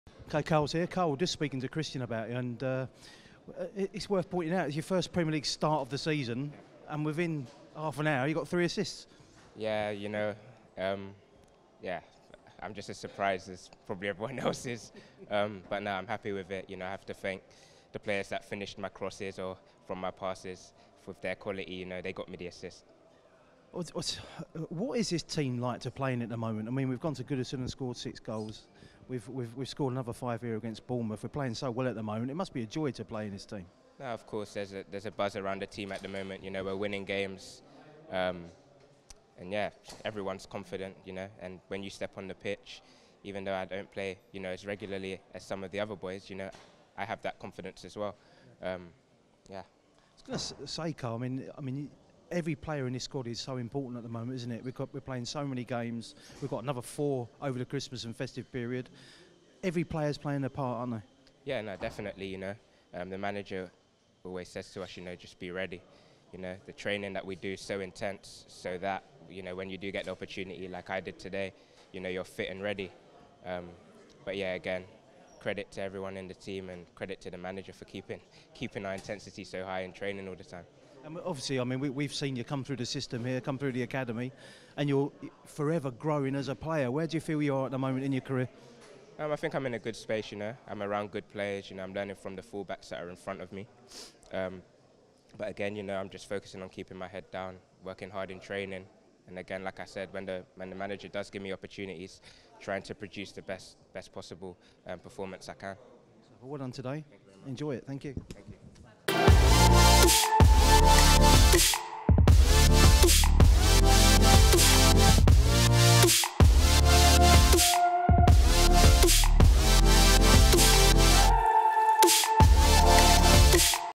THREE FIRST-HALF ASSISTS! Kyle Walker-Peters on Bournemouth win | Spurs 5-0 AFC Bournemouth
Hear from Kyle Walker-Peters as he grabbed three first-half assists on his first Premier League start of the season during Tottenham Hotspur's 5-0 win against Bournemouth.